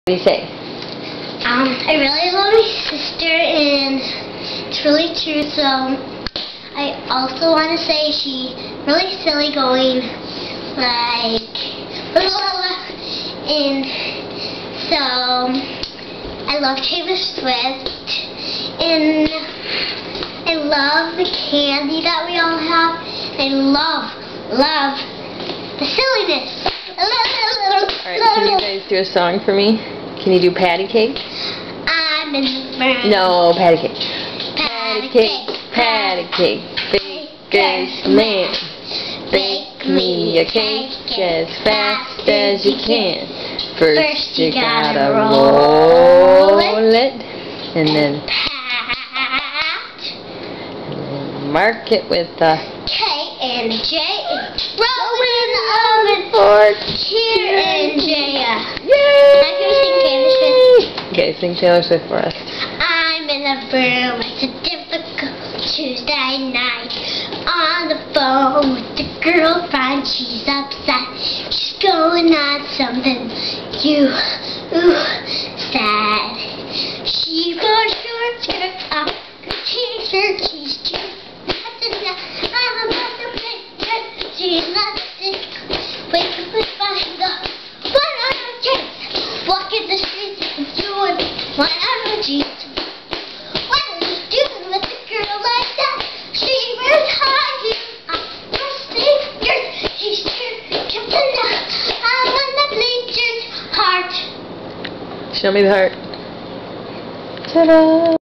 Jaya and Kirra Singing
Usually Kirra would do some more hand motions but she was into eating her banana.  She is like Jaya's back up dancer for the Taylor Swift part!  Got me laughing as you will see!